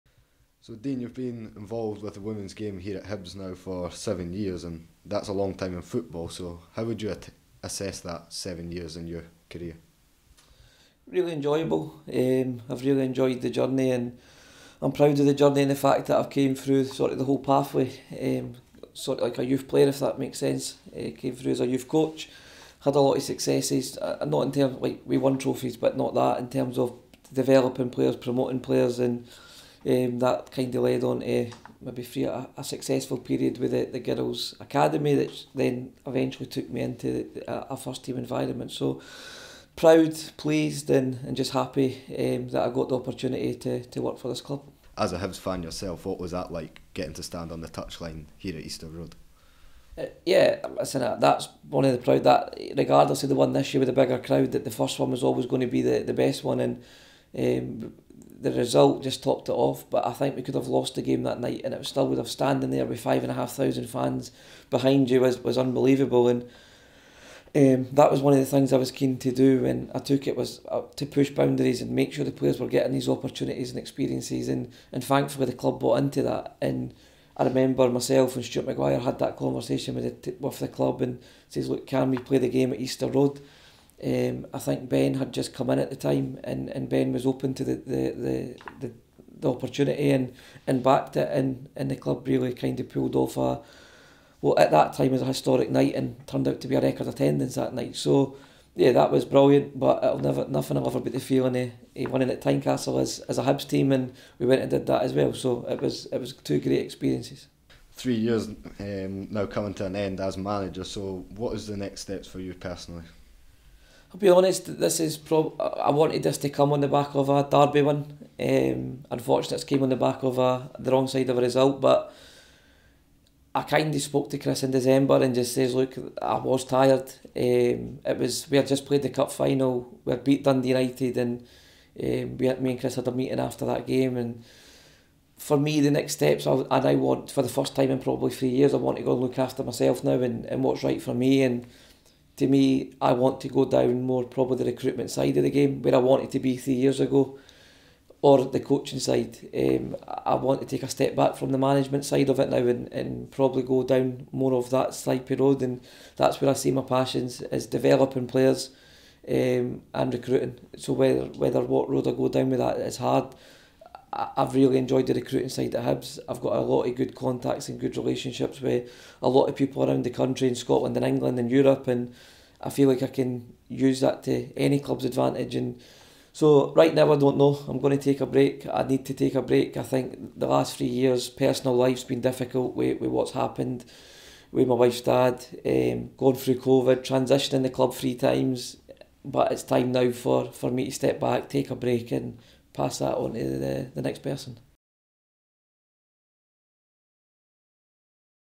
"I’ve Really Enjoyed The Journey" - Dean Gibson On His Time At Hibernian Women
Dean Gibson spoke exclusively to Hibs TV after it was announced that he will depart his role as Hibernian Women’s Manager at the end of the 2022/23 campaign.